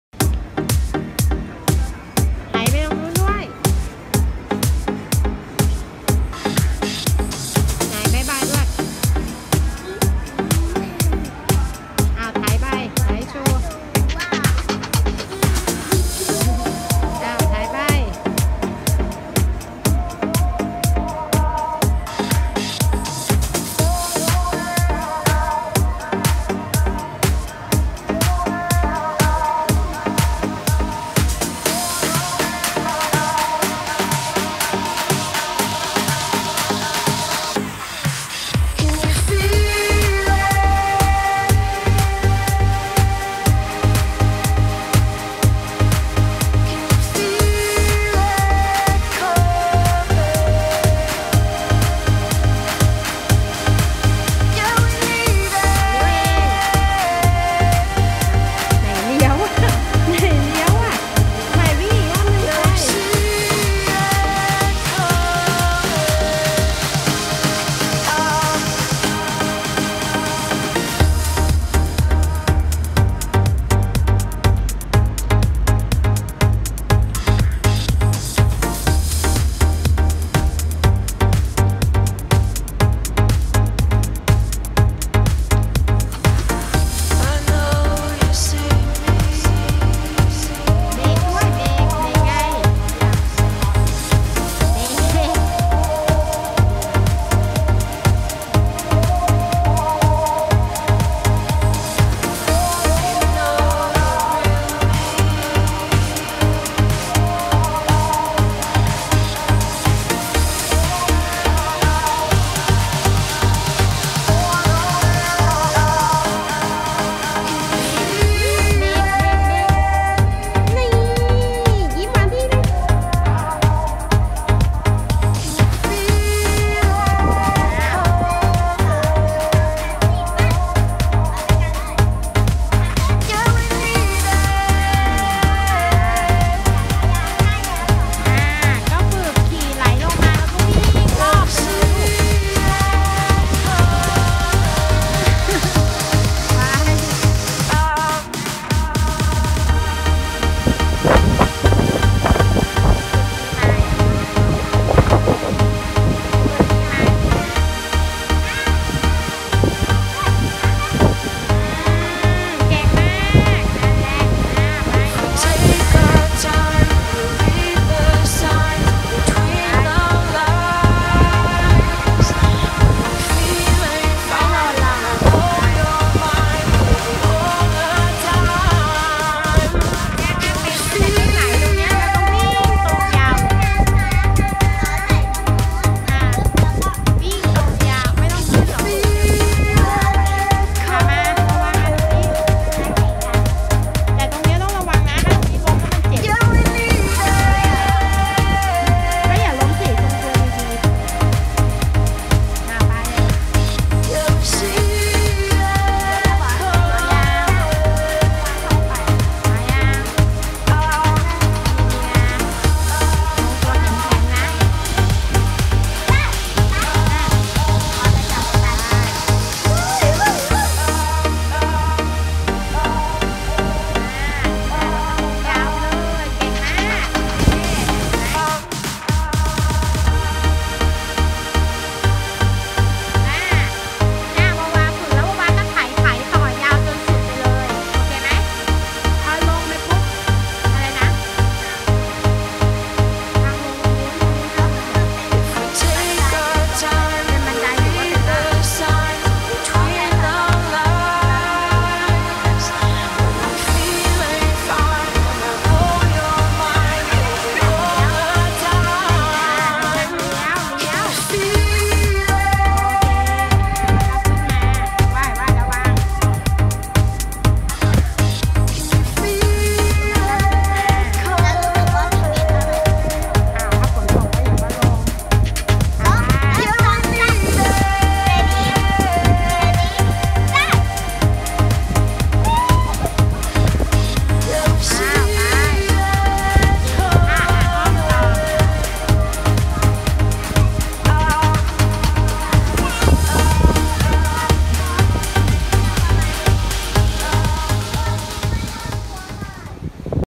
EP.8 วาวาพาเล่น Scooter & Balance Bike
กิจกรรมสนุกๆ เพิ่มความสุขและทักษะการทรงตัว